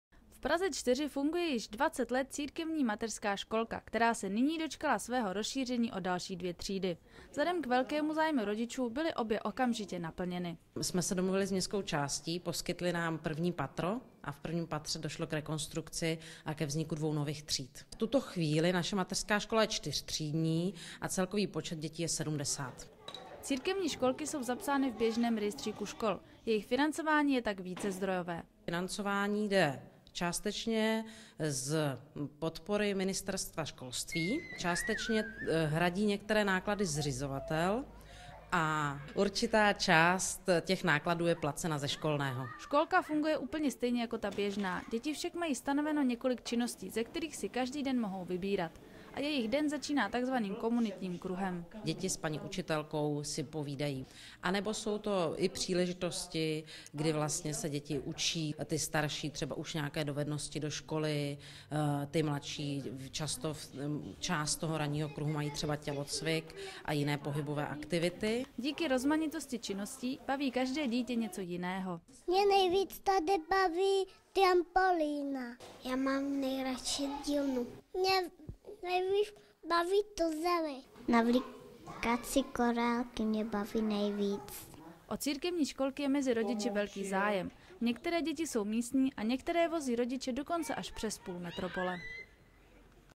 Církevní mateřská škola Studánka
Mnoho pražských rodičů předškolních dětí řeší problém s umístěním svých potomků do mateřských škol. S nedostatečnou kapacitou v Praze pomáhají do určité míry i mateřské školy církevní. Ta v Praze 4 se nyní dočkala rozšíření o další dvě třídy.

Televize Metropol je pražská televize, která přináší aktuální zpravodajství z Prahy, kulturní tipy a spoustu dalších informací z hlavního města.